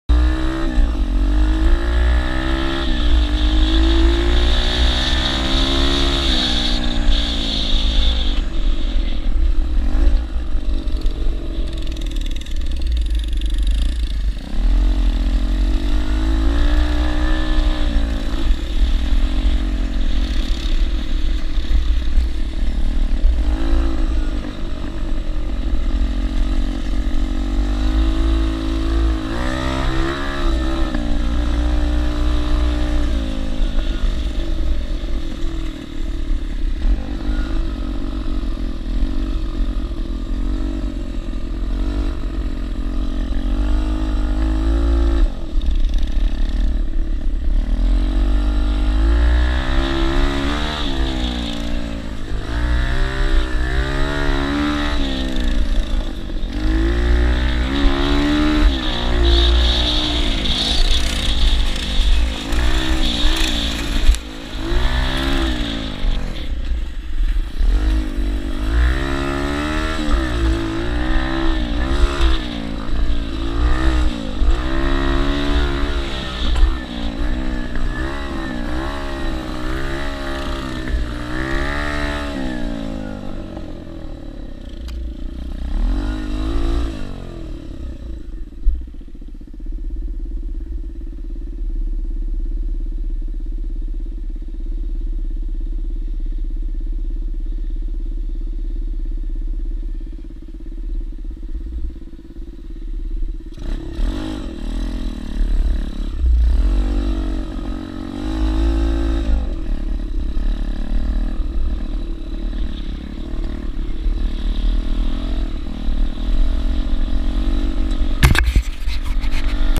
APMP ATV BigRed JRR 26th
12/17/21 - Quick run up the road with the family on off-road stuff.